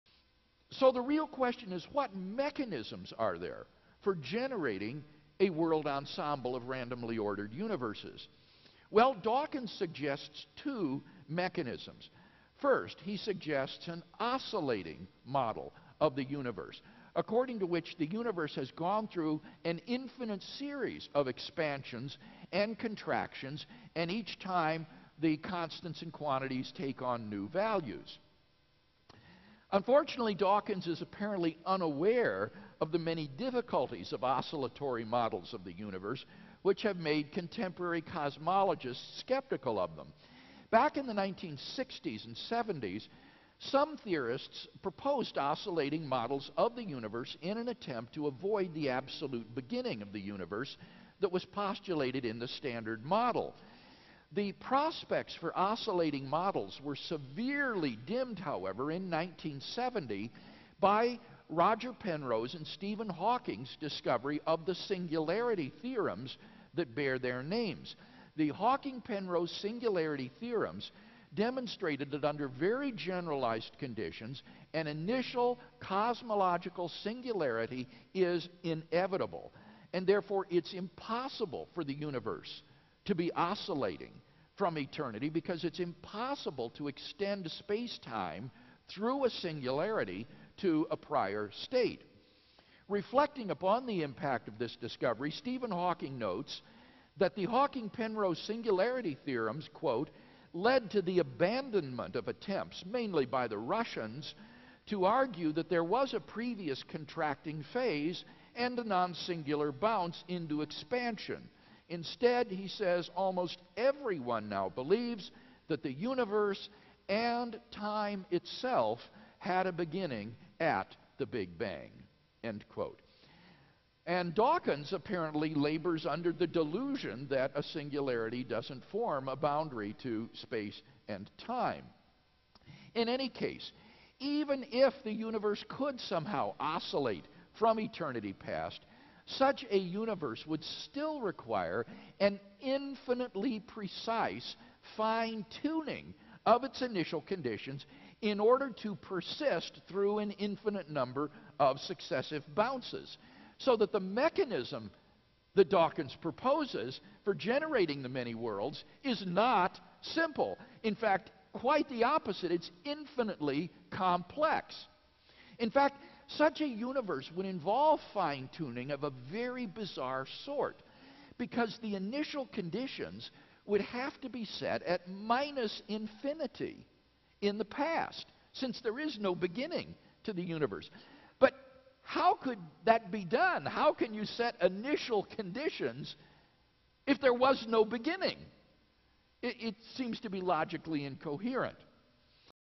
Richard Dawkins' Ignorance of the Oscillating Universe
- Dr. William Lane Craig exposes Richard Dawkins' (author of The God Delusion) ignorance of current cosmology and the oscillating model.

The following video clip comes from Craig's refutation of Dawkins on the Teleological argument for God